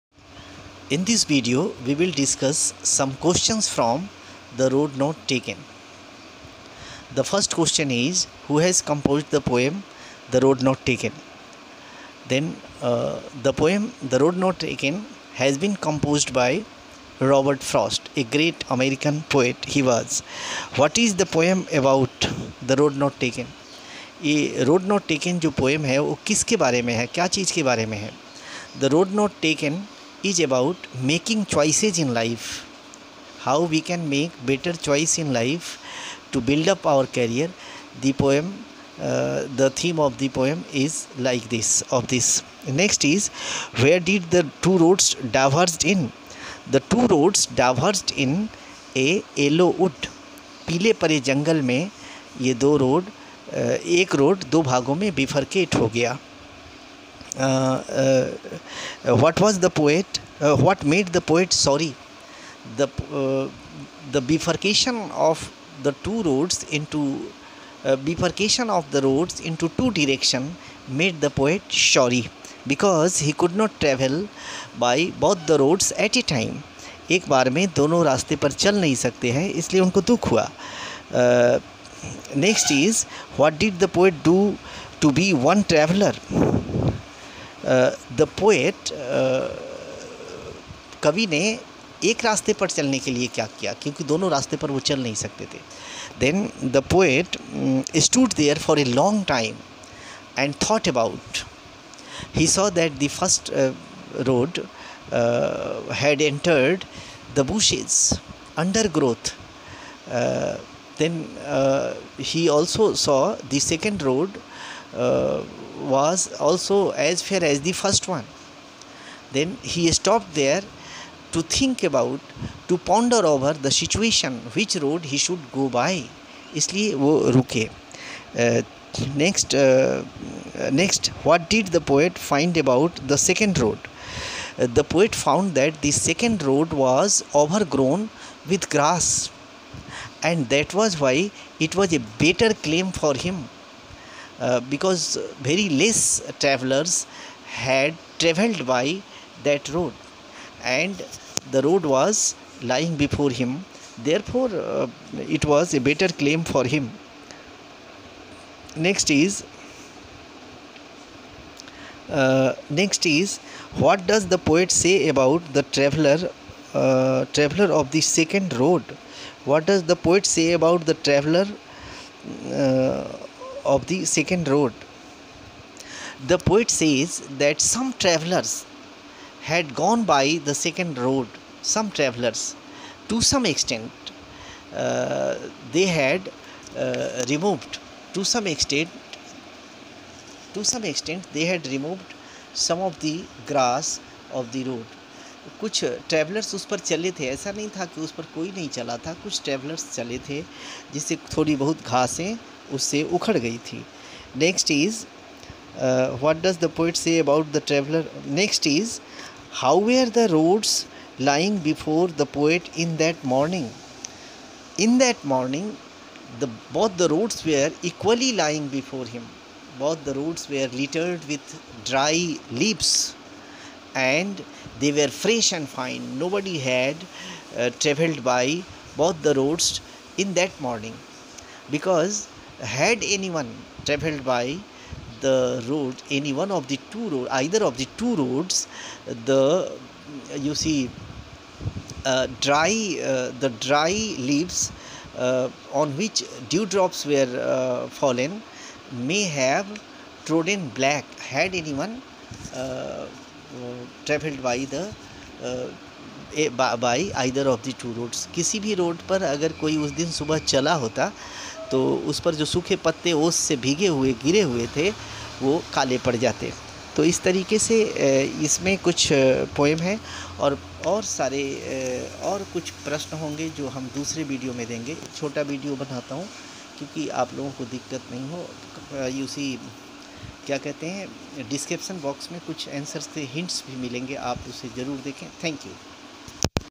Questions/Answers from the Road Not Taken by Dinesh Sir.
Hints:- a) Who has composed the poem"The Road Not Taken.
Ans:- Robert Frost.
b) What is the poem " The Road Not Taken " about?
Ans:-The poem "The Road Not Taken" is about making right Choice in life .
c) What made the poet sorry?
Ans:- The bifurcation of the road into two directions made the poet sorry as he couldn't travel by both the roads at a time .
d) What did the poet do to be one traveller?
Ans:- To be one traveller the poet stood there for a long time and examined the two roads .